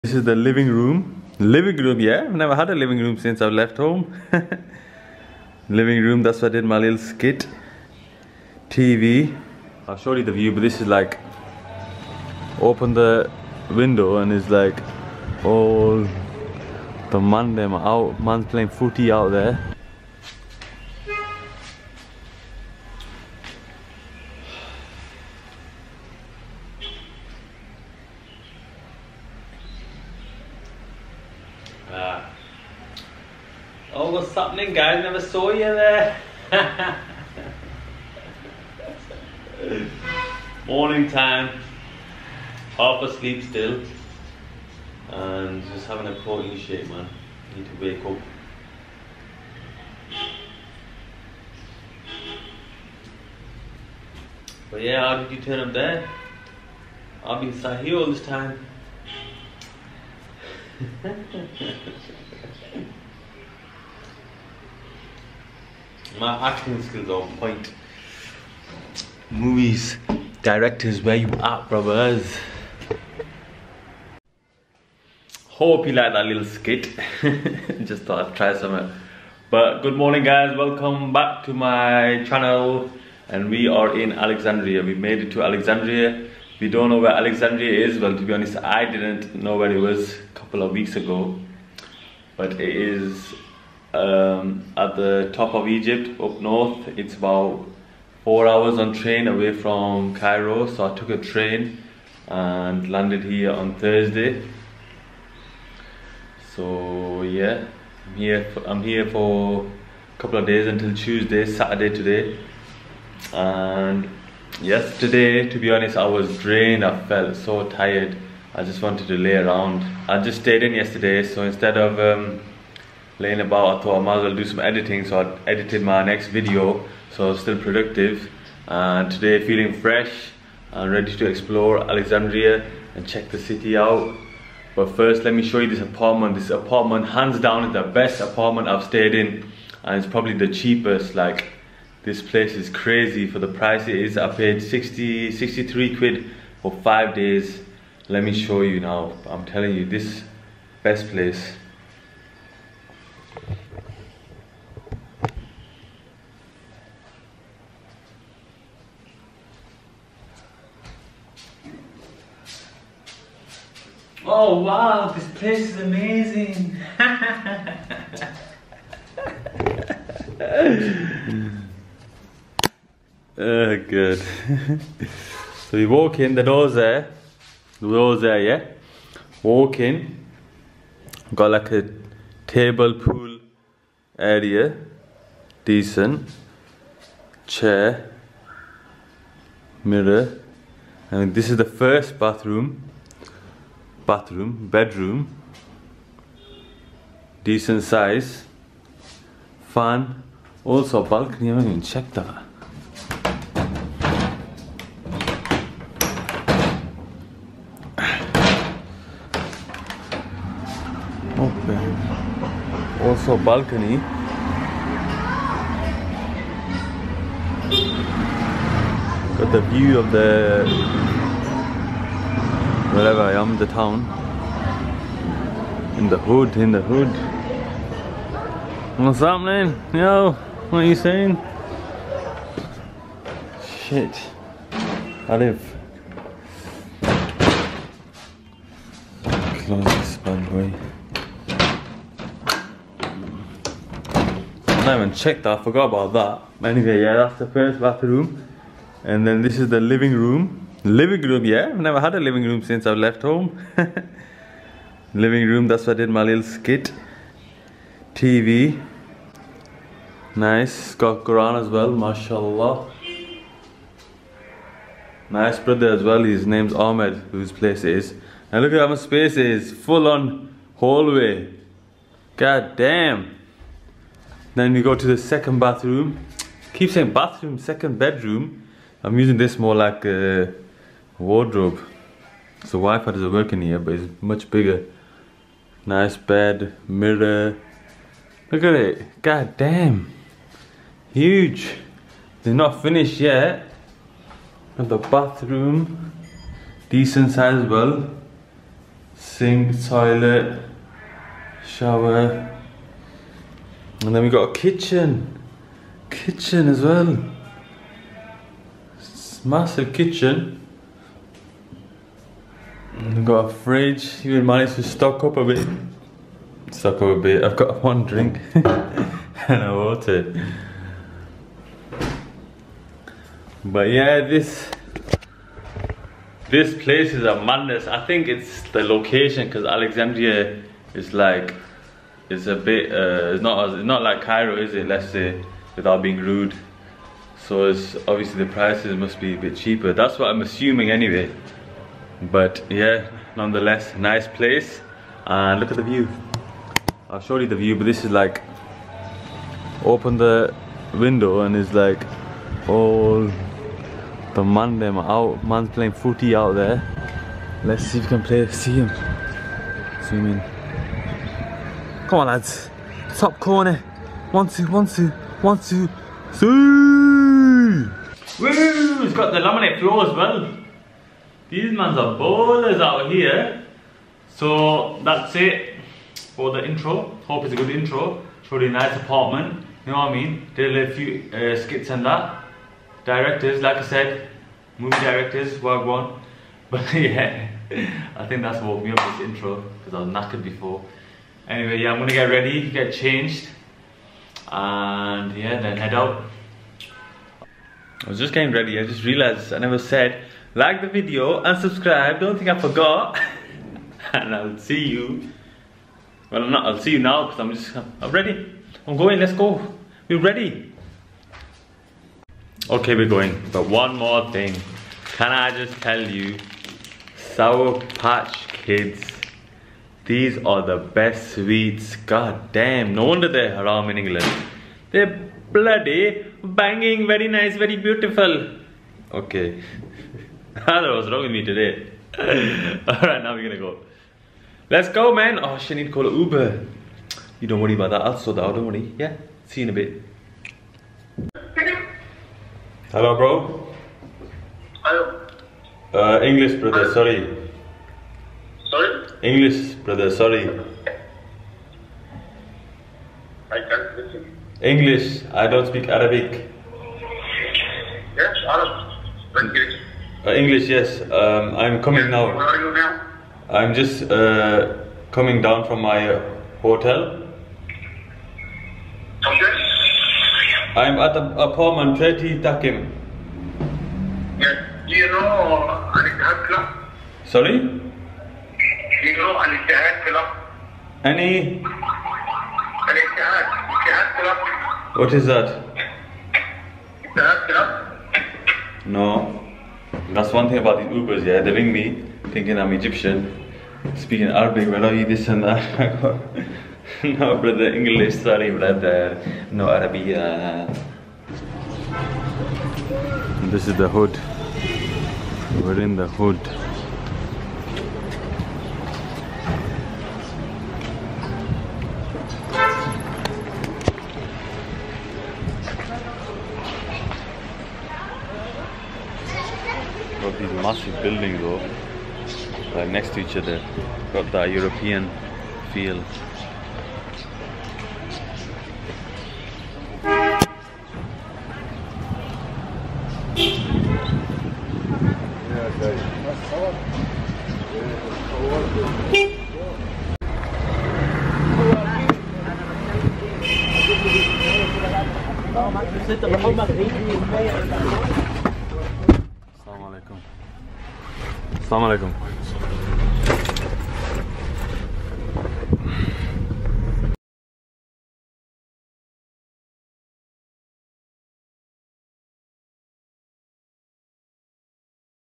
$15 A NIGHT APARTMENT IN ALEXANDRIA EGYPT 🇪🇬 - الشقة لخمسة عشرة دولار في الاسكندري
Today we moved cities and ended up in Alexandria! My first time here , and i was blown away with the apartment i was staying at! The pictures looked okay and I wasn’t expecting much but when i got there it looked so much better! Especially with the price i paid for , i paid roughly $15 a night which is so cheap for a place like this. So i thought id give you a tour of the apartment and the area on where im staying.

This is one of many videos from my youtube vlog series about my time in Egypt, you can find the entire playlist